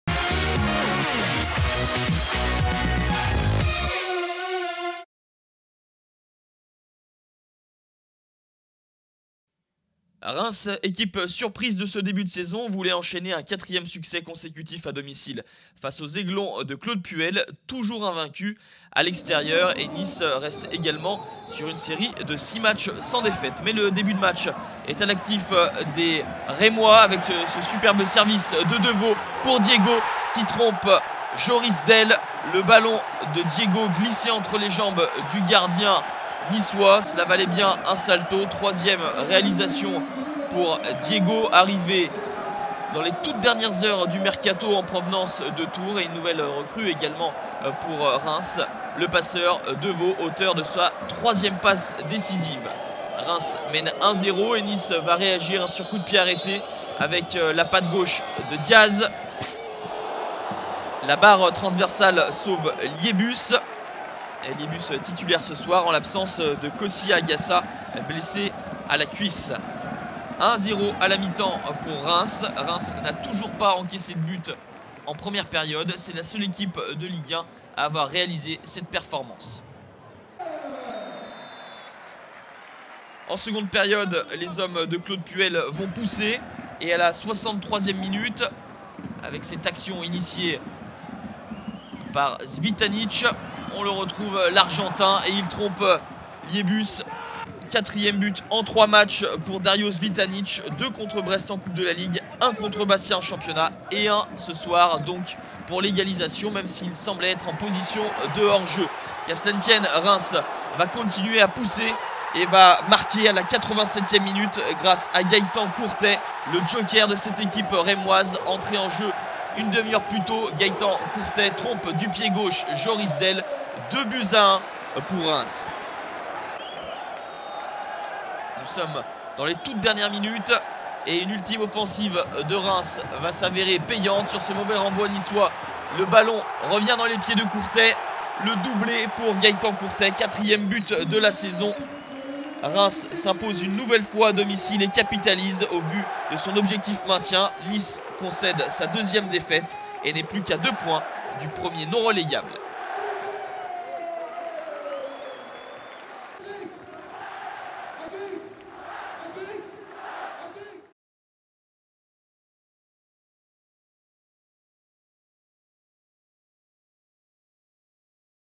Stade de Reims - OGC Nice (3-1) - Le résumé (SdR - OGCN) / 2012-13
Revivez les meilleurs moments de Stade de Reims - OGC Nice (3 - 1) en vidéo.

Stade Auguste Delaune - samedi 6 octobre 2012

Buteurs : Diego RIGONATO (16' - Stade de Reims) / Dario CVITANICH (62' - OGC Nice) / Gaëtan COURTET (87' - Stade de Reims) / Gaëtan COURTET (90' +3 - Stade de Reims)

Composition Stade de Reims : 1 - Johan LIEBUS / 4 - Grzegorz KRYCHOWIAK / 6 - Antoine DEVAUX / 11 - Diego RIGONATO / 13 - Nicolas FAUVERGUE / 21 - Bocundji CA / 22 - Mickaël TACALFRED (c) / 23 - Aissa MANDI / 25 - Anthony WEBER / 26 - Pape Ndiaye SOUARE / 27 - Christopher GLOMBARD

Composition OGC Nice : 2 - Renato CIVELLI / 4 - Nemanja PEJCINOVIC / 6 - Didier DIGARD (c) / 8 - Mahamane TRAORE / 12 - Dario CVITANICH / 13 - Valentin EYSSERIC / 14 - Jérémy PIED / 19 - Kévin DIAZ / 21 - Lloyd PALUN / 25 - Romain GENEVOIS / 30 - Joris DELLE